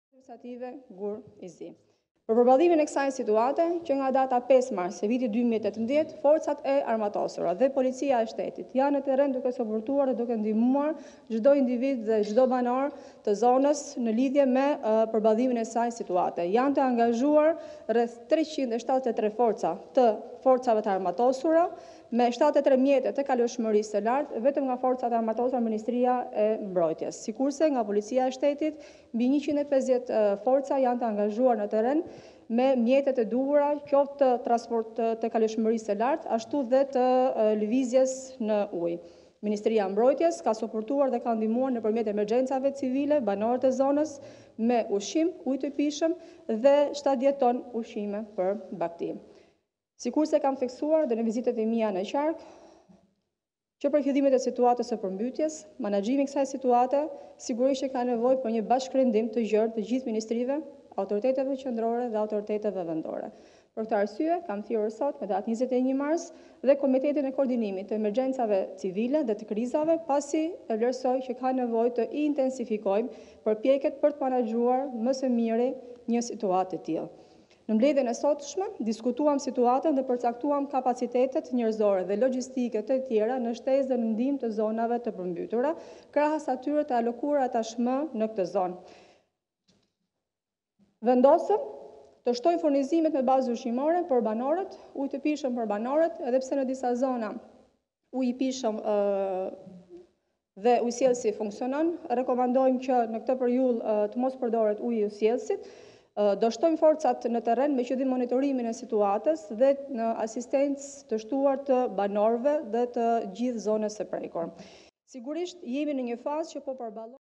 Ora News - Qeveria nuk shpall emergjencën në Shkodër, Mesi: Do shtojmë forcat dhe ndihmat
Përkundër kërkesës së përsëritur të Bashkisë së Shkodrës, duket se qeveria mbetet e vendosur për të mos shpallur gjendjen e pjesshme të emergjencës në zonat e prekura nga përmbytjet.

Pas mbledhjes së Këshillit të Ministrave, numri dy i qeverisë Senida Mesi teksa evidentoi faktin se kryeqendra e veriut prej rreth 3 javësh po "lëngon" nën ujë, lajmëroi shtimin e forcave në terren dhe ndihmave ushqimore por edhe ujit të pijshëm për banorët.

Mesi: Në mbledhjen e sotme përcaktuam kapacitet njerëzore, logjistike etj shtesë në ndihmë të zonave të përmbytura.Vendosëm të shtojmë furnizimet me bazë ushqimore për banorët, ujë të pijshëm. Edhe pse në disa zona Ujësjellësi funksionon rekomandojmë që në të këtë periudhë të mos përdoret uji i tij. Do të shtojmë forcat në terren.

Mesi shtoi më tej se në fazën e rikuperimit është shumë e rëndësishme të vlerësojmë dëmet, pasojat dhe të marrim masat për parandalim të mëtejshëm në vazhdim.